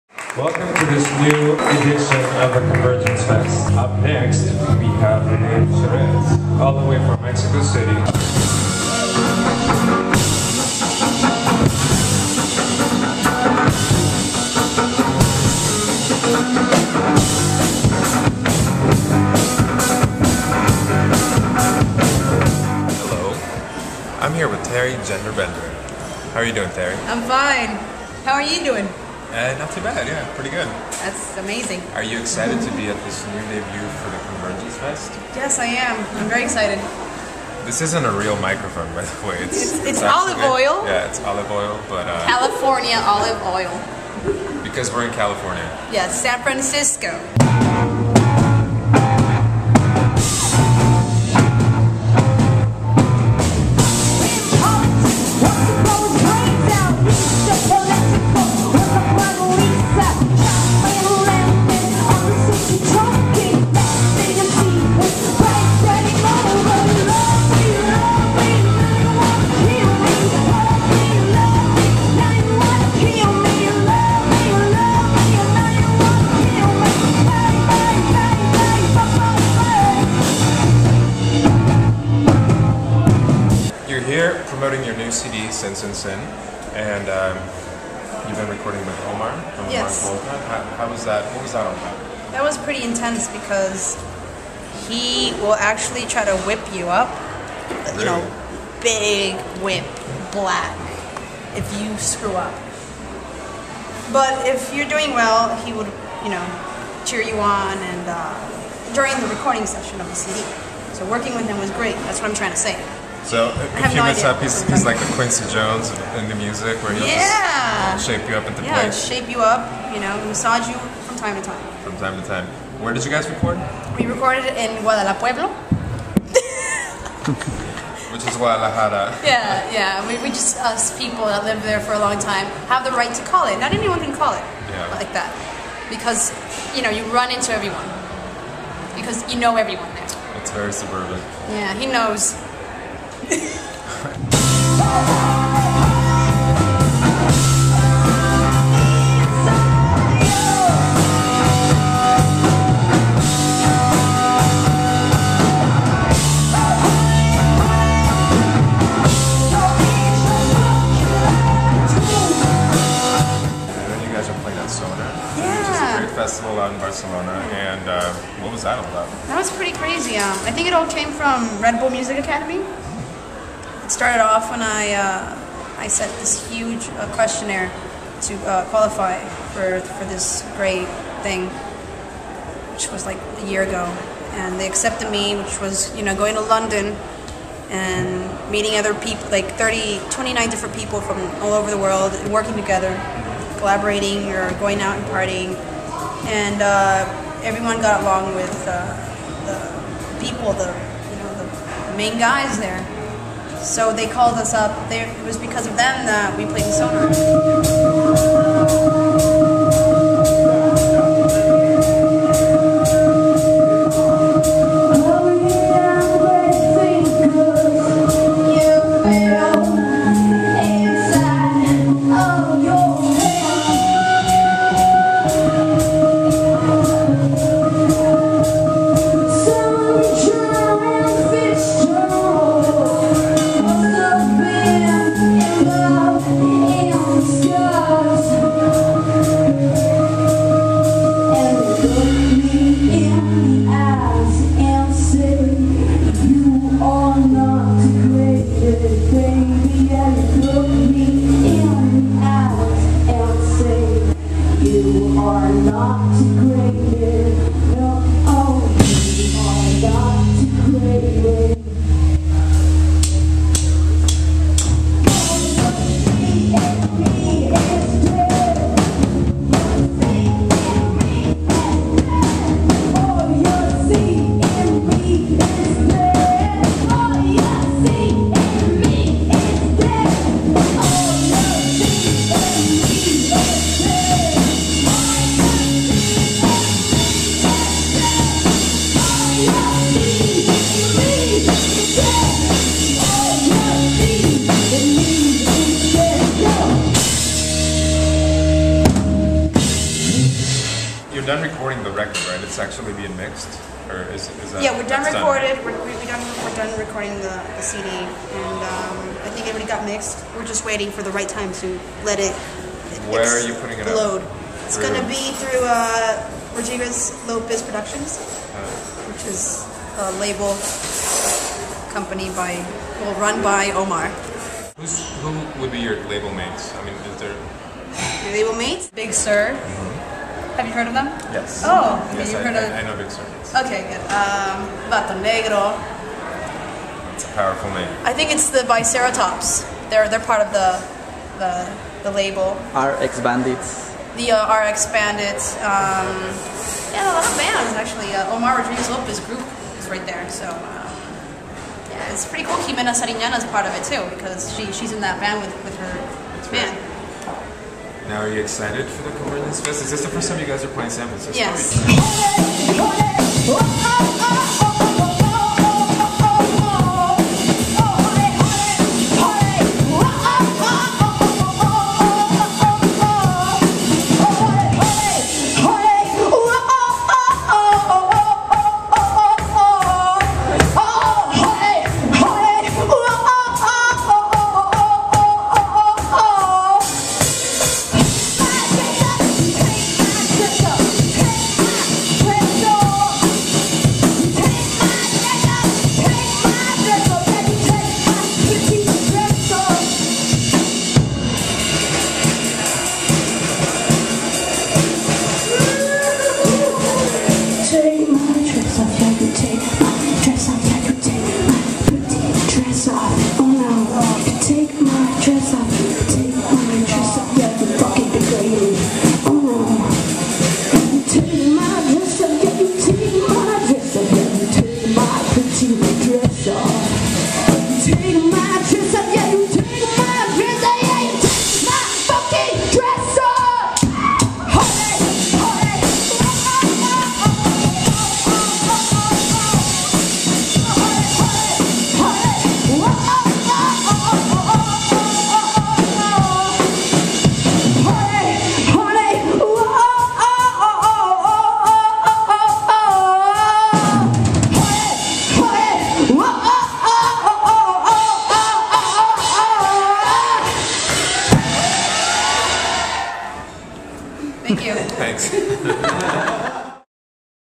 Marcelo C. Baez Interviews Teri Gender Bender - Le Butcherettes
DJ, chanteur and bon vivant, Marcelo C. Baez interviews Teri Gender Bender at the CONVERGENCE MMX Preview Party - July 17, 2010